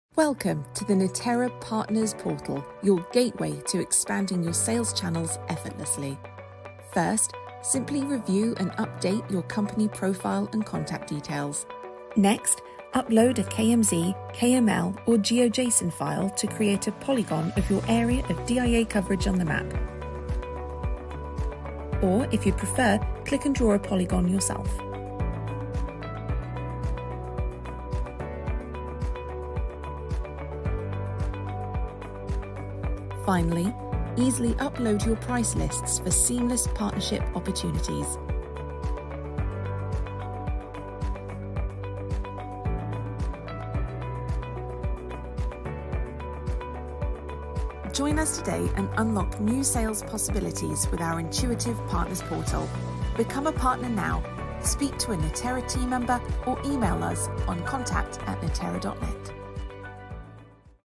Neterra Partners' Portal
🌐 Neterra’s Partners Portal enables you to easily review and update your company profile and contact details, to upload a kmz, kml, or geo-json file to create a polygon of your area of DIA coverage on the map. 🗺️✨

📄 You can also easily upload your price lists for seamless partnership opportunities.

🔓 Join us today and unlock new sales possibilities with our intuitive partners' portal.

🤝 Become a partner now!